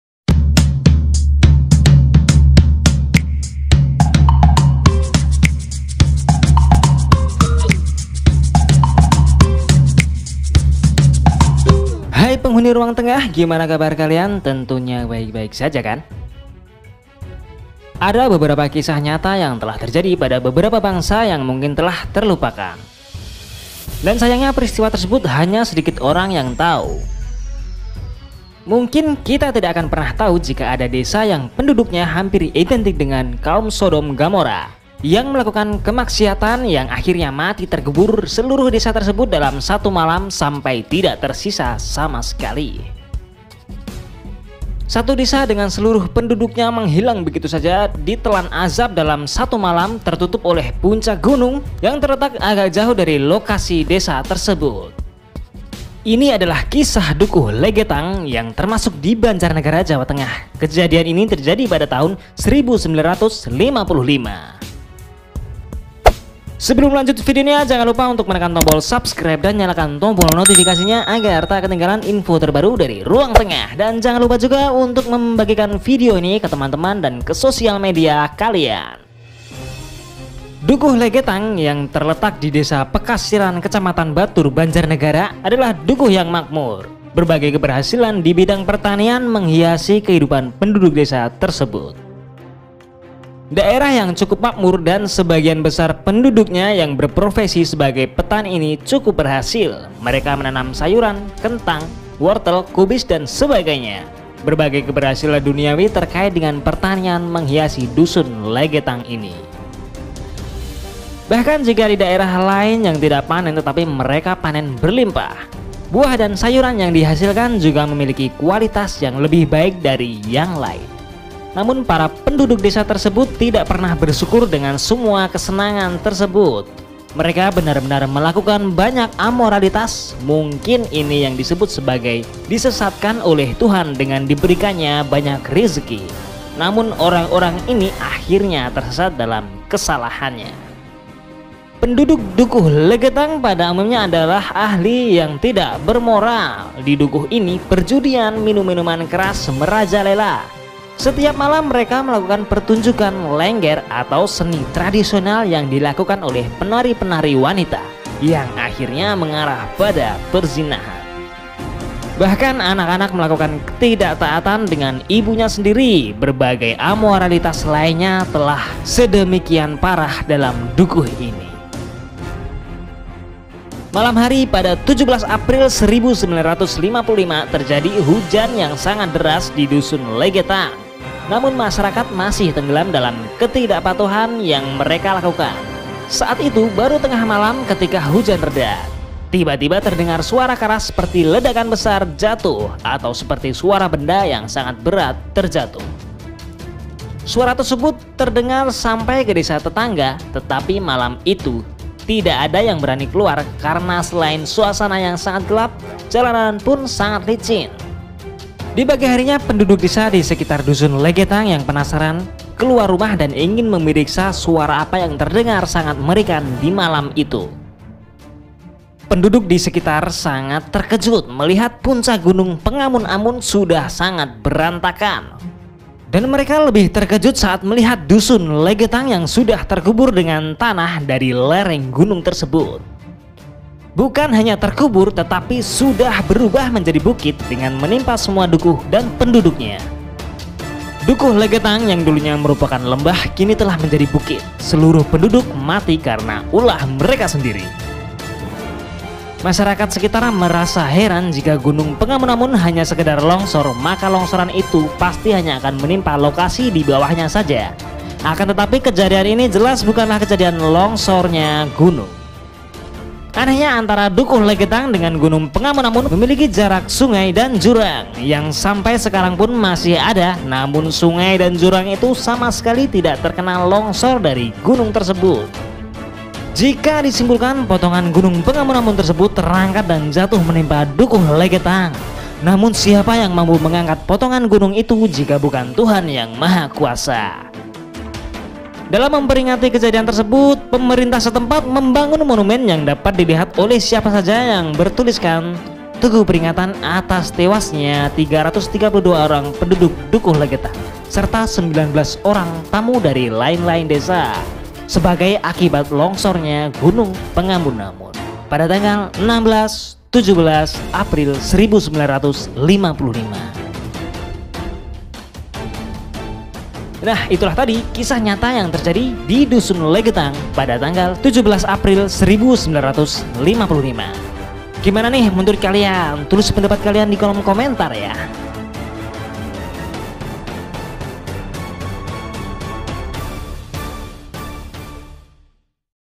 ADA DI INDONESIA | Desa Ini Lenyap Dalam Semalam | Kisah Dukuh Legetang Banjarnegara
Ada beberapa kisah nyata yang telah terjadi pada beberapa bangsa yang mungkin telah terlupakan. Dan sayangnya, peristiwa tersebut hanya sedikit orang yang tau

.mungkin kita tidak akan pernah tahu jika ada desa yang penduduknya hampir identik dengan kaum Sodom-Gomora, yang melakukan kemaksiat, yang mati terkubur seluruh desa tsb dalam satu malam sampai tidak tersisa sama sekali.

Satu desa dengan seluruh penduduknya menghilang begitu saja di telan azab dalam satu malam tertutup oleh puncak gunung yang terletak agak jauh dari lokasi desa

Video hanyalah ilustrasi